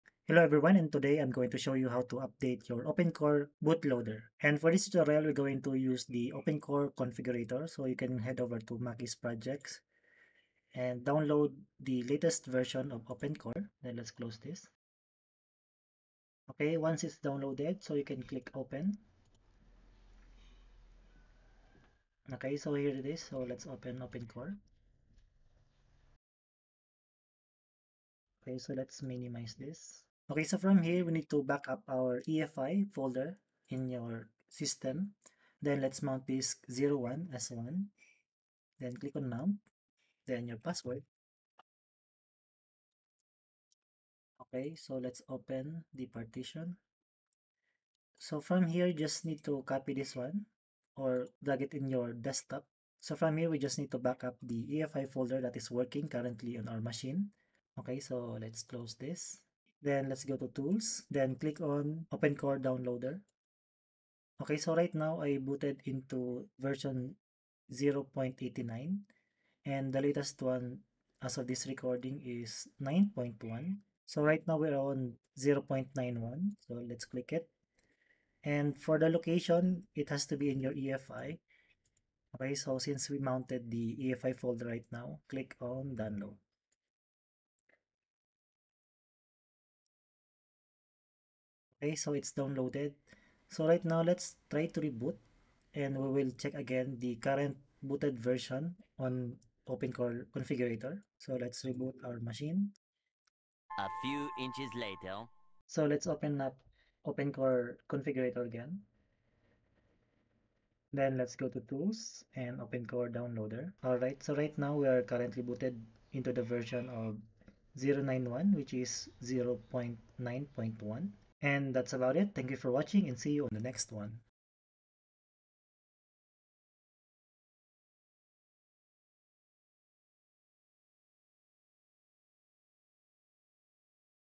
Hackintosh - Updating OpenCore Bootloader version 0.9.1
Hi Guys, this is a quick and detailed tutorial on how to update the OpenCore Bootloader of your Hackintosh Machine, I'm on Dell Optiplex 3080 with MacOS Ventura.

Should You Hackintosh in 2022 ? Not the Answer You Expect !
HACKINTOSH PERFEITO!!! VEJA COMO INSTALAR O MacOS MONTEREY NO LENOVO LEGION 5I !!!